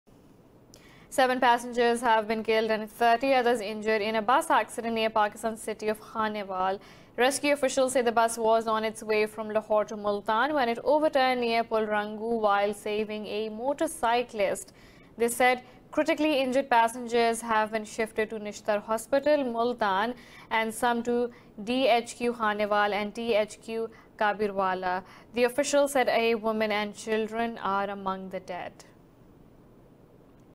Pakistan: 7 killed, 30 wounded in road accident | News Bulletin | Indus News
7 passengers have been killed, and 30 others injured, in a BusAccident near Pakistan’s city of Khanewal.